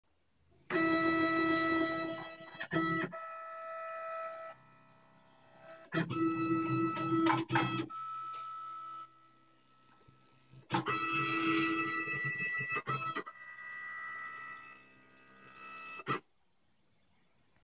Mini mill homing cycle
G28.2 homing cycle in TinyG with the newly-installed limit switches.